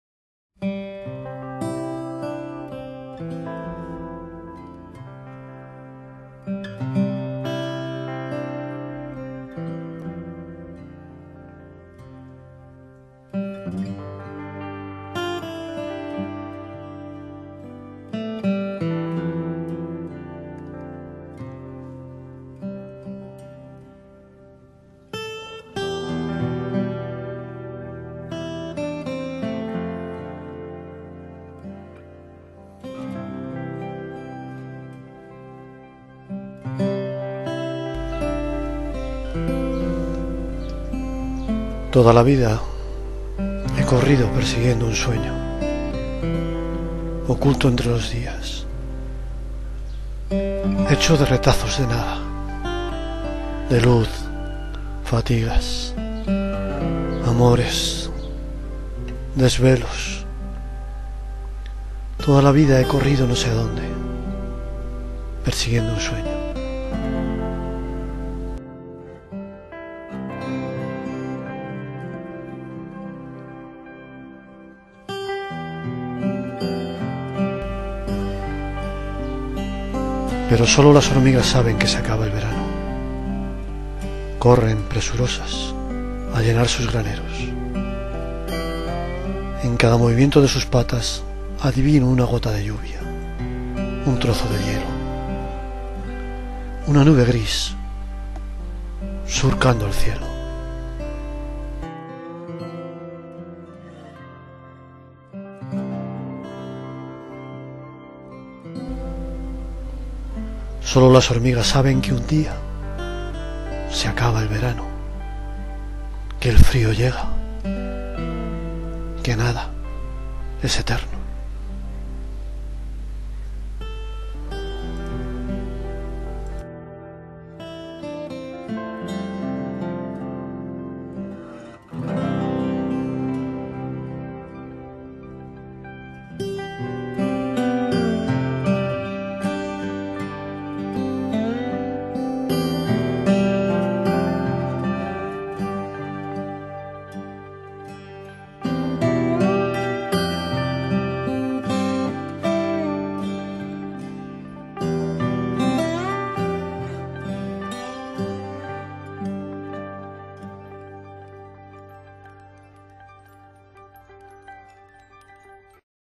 Solo las hormigas saben
Un poema de Miguel Guinea recitado por el autor